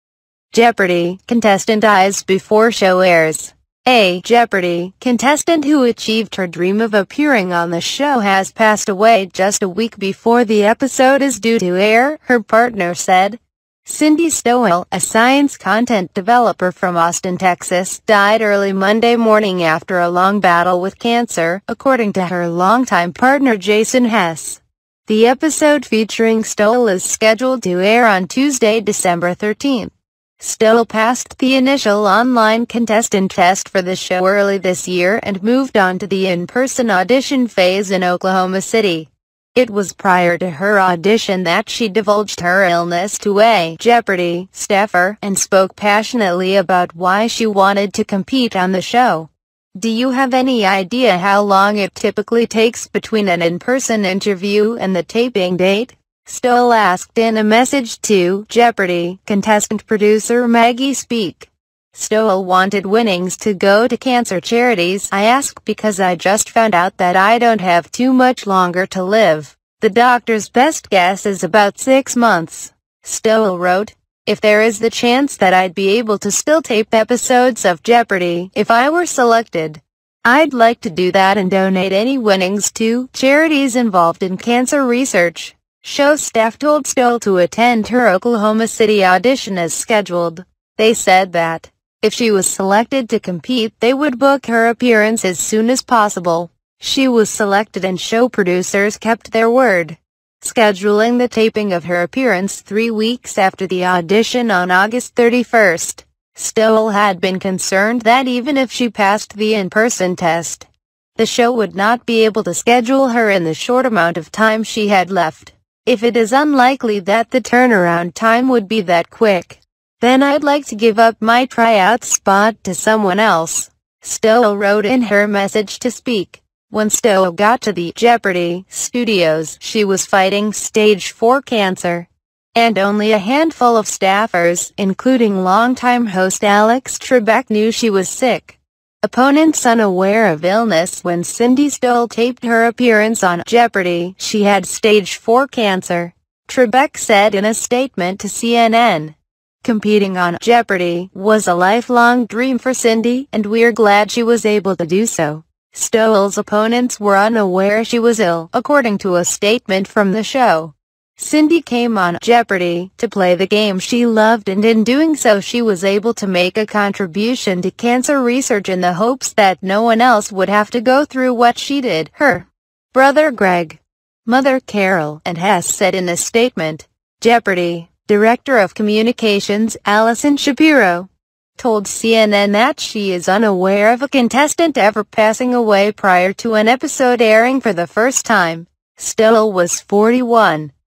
Jeopardy - contestant dies before show airs - Celebrity News #1
Jeopardy - contestant dies before show airs update
A “Jeopardy!” contestant who achieved her dream of appearing on the show has passed away just a week before the episode is due to air, her partner said.

Cindy Stowell, a science content developer from Austin, Texas, died early Monday morning after a long battle with cancer, according to her longtime partner Jason Hess. The episode featuring Stowell is scheduled to air on Tuesday, December 13.